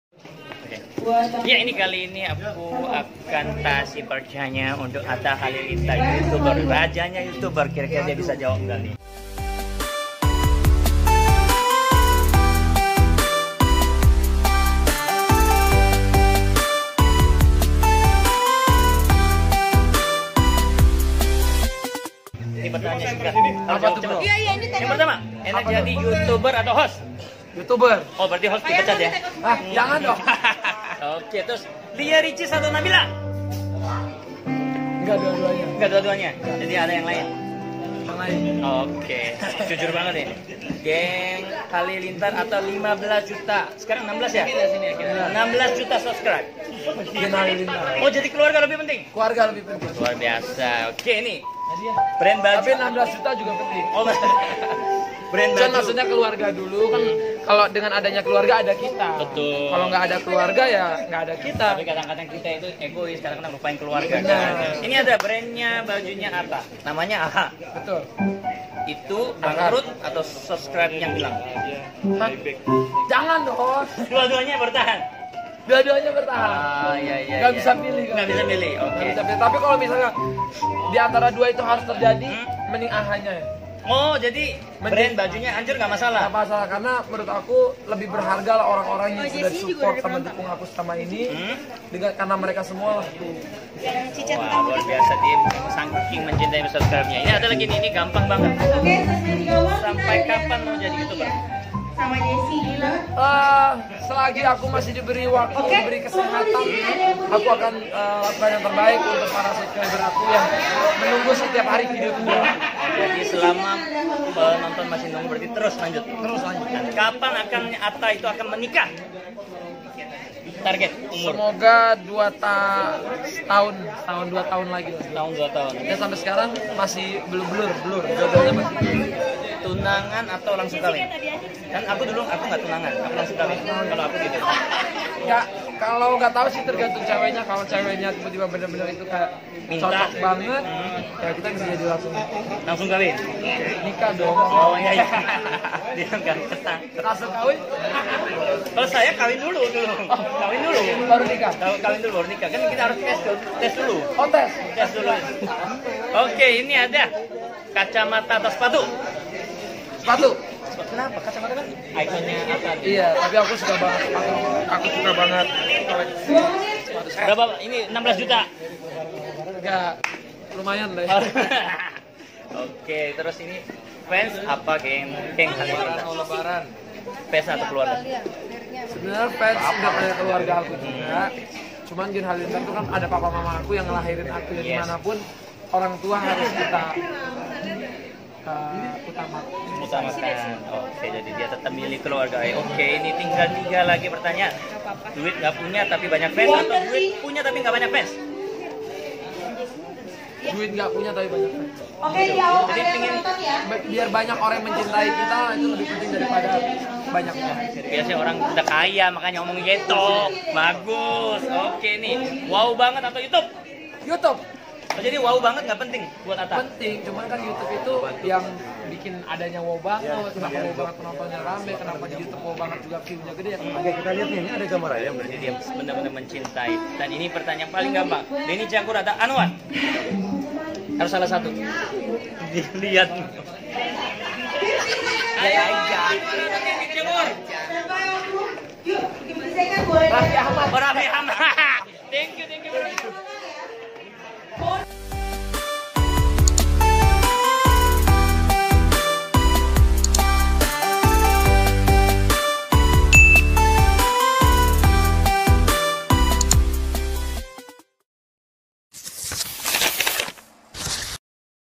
ATTA SIAP NIKAH DAN KELUAR DARI WOW BANGET!!!
Anyeonghaseo... kali ini aku lagi bareng sama Atta Halilintar. Seru banget belum lama ini aku syuting sama dia. Kolaborasi pertama aku sama dia, sudah bongkar rahasia pribadi nih. Atta ternyata siap nikah, dan siap keluar dari Wow Banget Trans7. Beneran? So, simak segera chit-chat saya sama Atta. Sukses selalu Atta! Terimakasih.

Watermelon !!! 좋은 하루 보내세요! :) ㅎㅎ